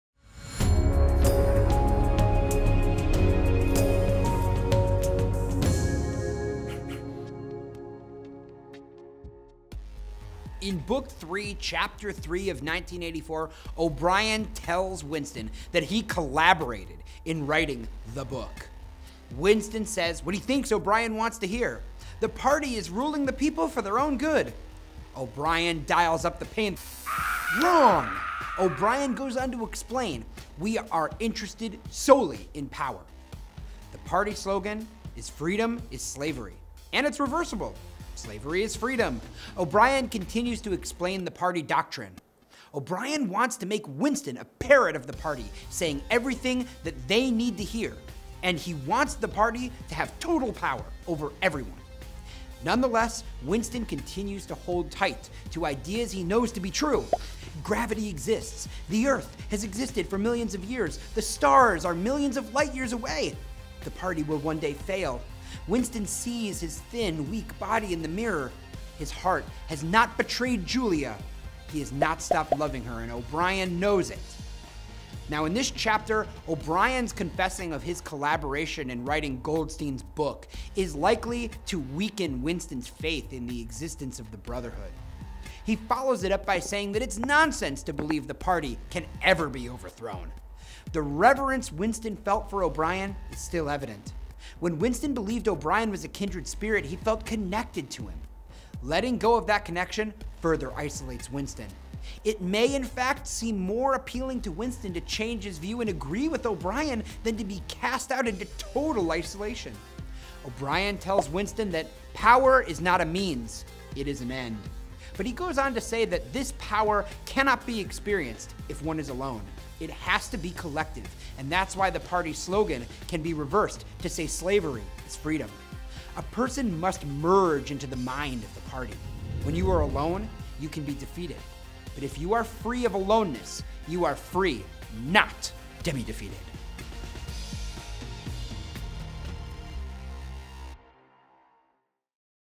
1984 | Book 3 | Chapter 3 Summary & Analysis | George Orwell
Summarize videos instantly with our Course Assistant plugin, and enjoy AI-generated quizzes 1984 Book 3, Chapter 3 summary and analysis in under five minutes!  George Orwell's classic dystopian science fiction novel 1984 deals with a totalitarian future in which speech, privacy and thought is all totally controlled by the government.  This video provides an in-depth summary and analysis of Book Three, Chapter Three of George Orwell's classic book.

Download the free study guide and infographic for George Orwell's novel 1984